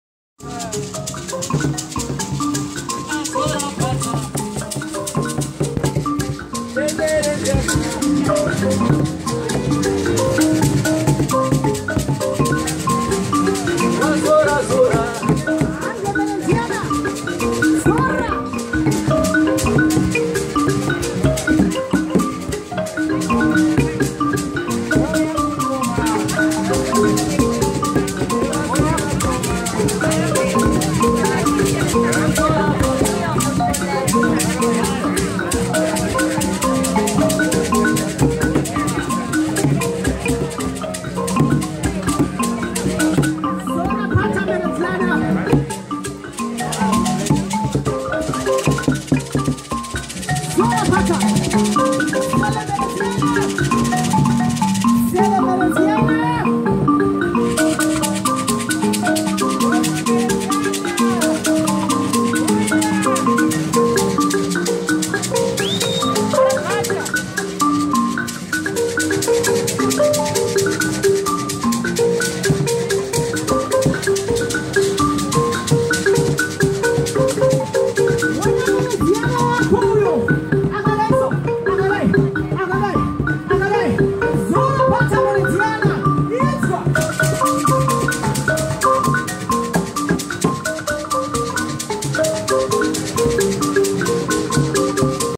GIANT PUPPET @ HIFA 2017 HARARE ZIMBABWE DANCING TO MBIRA BY KELVIN CHIKUMBIRIKE AND TIRIKOTI ARTS
GIANT PUPPETS MADE BY HANDS USING RECYCLED MATERIALS.
BOOKER AND TIRIKOTI ARTS